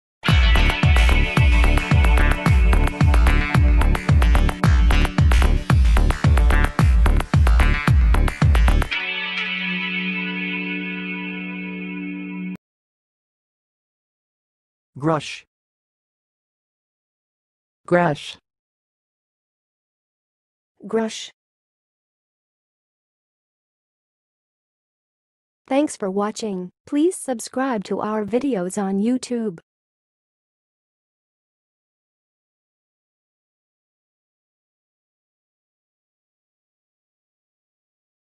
How to say "ghrush"! (High Quality Voices)
Watch in this video how to say and pronounce "ghrush"!